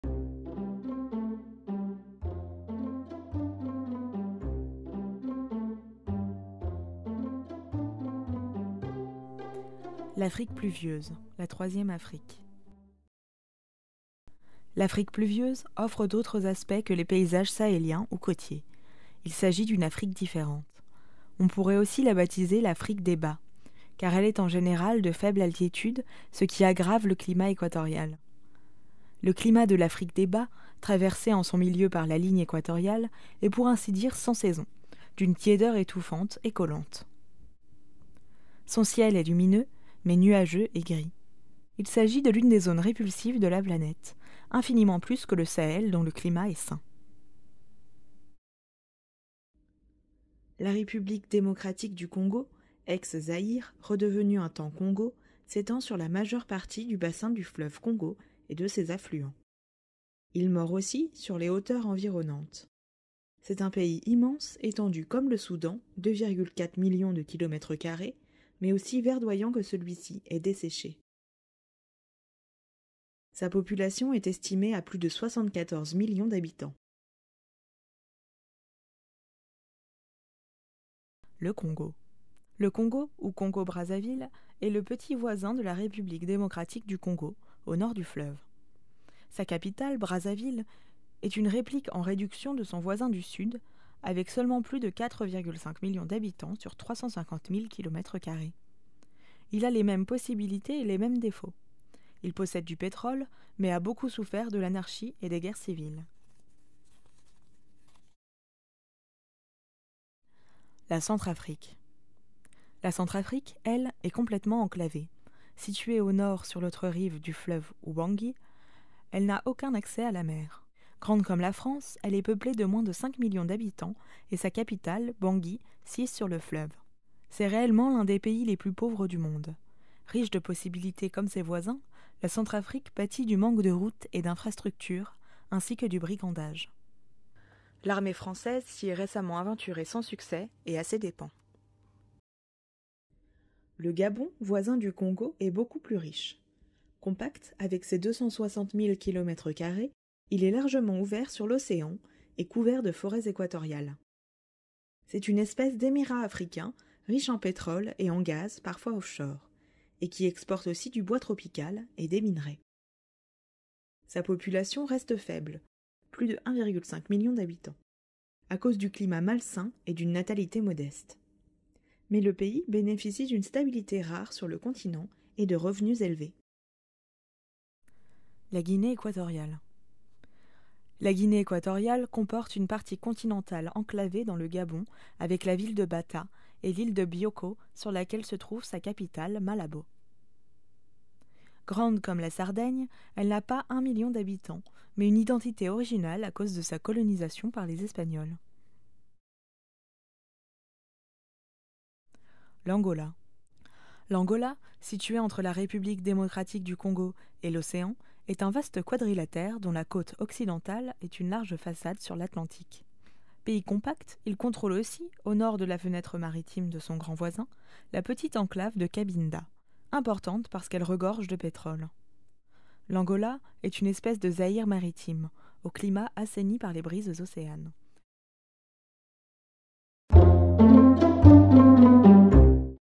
TOUTE LA GÉOGRAPHIE DU MONDE # La masse impénétrable de l'Afrique - 3 l'Afrique des hauts
D'après le texte, Toute la géographie du monde, Fayard
Jean Claude Barreau et Guillaume Bigot